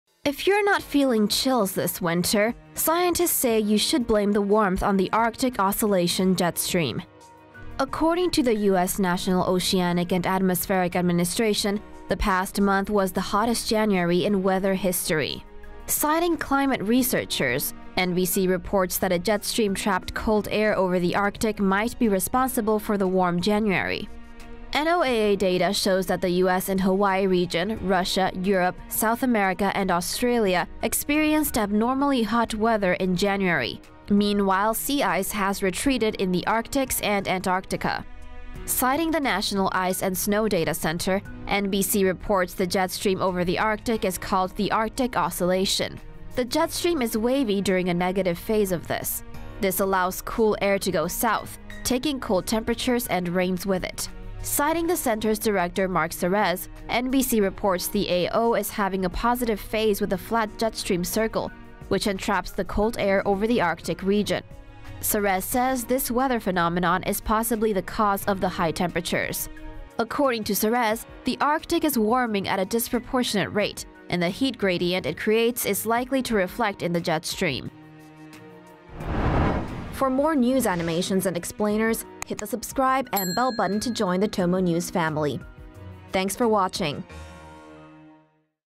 Why January 2020 was the warmest in history- TomoNews
BOULDER, COLORADO — The past month was the hottest January in weather history, according to the U.S. National Oceanic and Atmospheric Administration.

SOURCES: National Oceanic and Atmospheric Administration (NOAA), NBC News